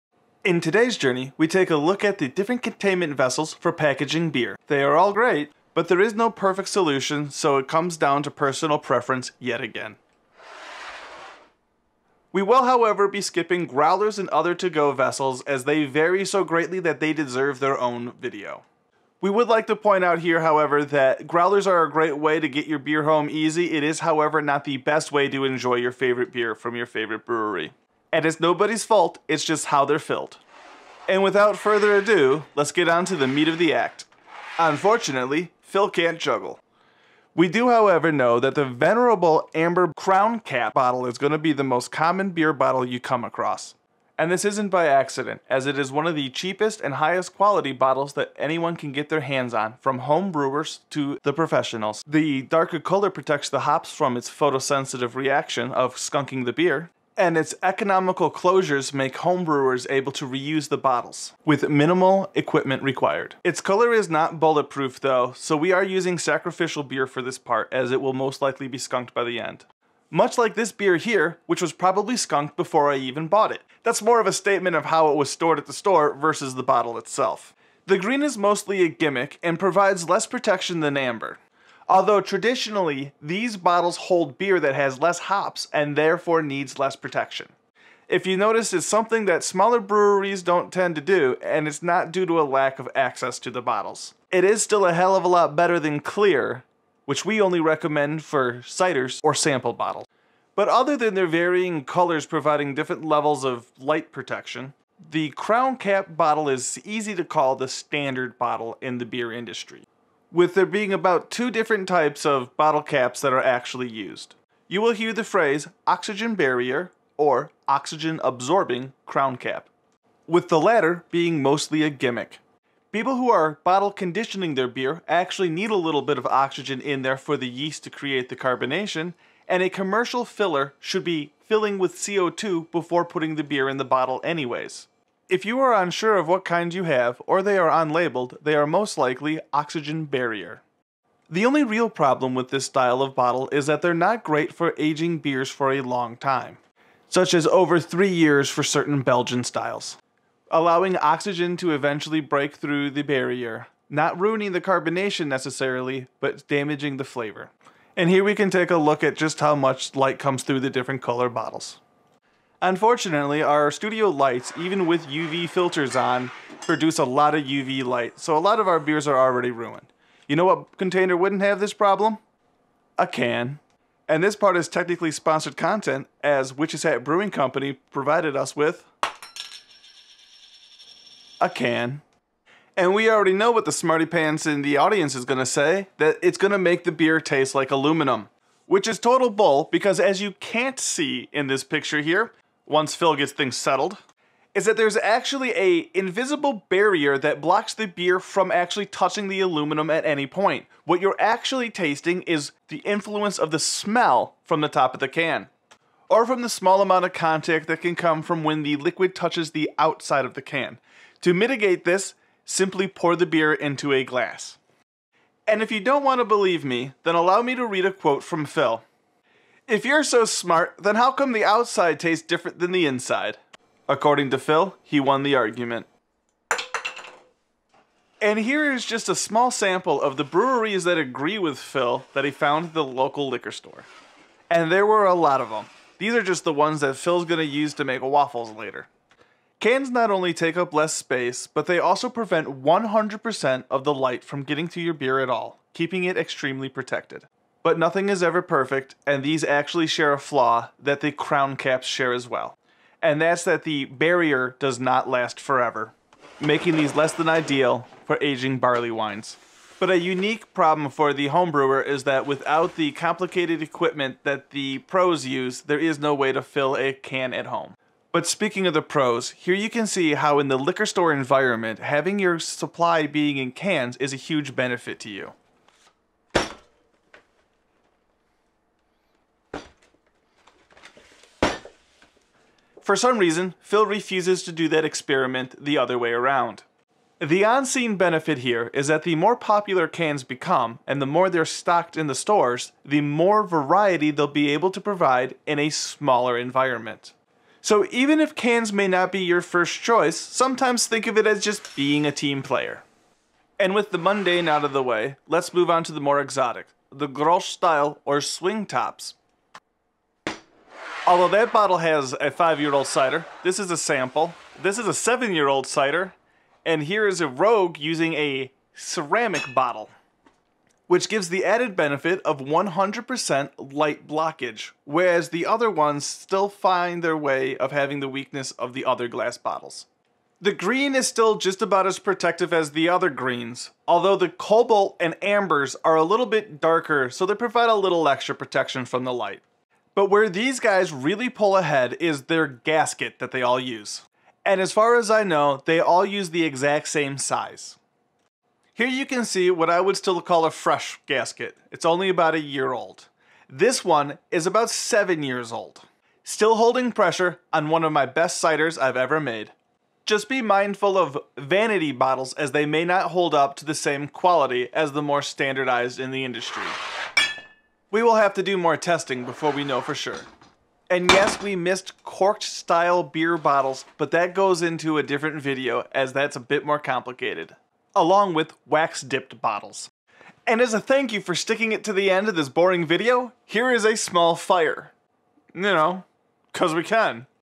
Basics of Bottles
In today's Journey, we go over some of the basic beer containment vessels that you will most likely come across in your day to day life. A special surprise to those that wait till the end.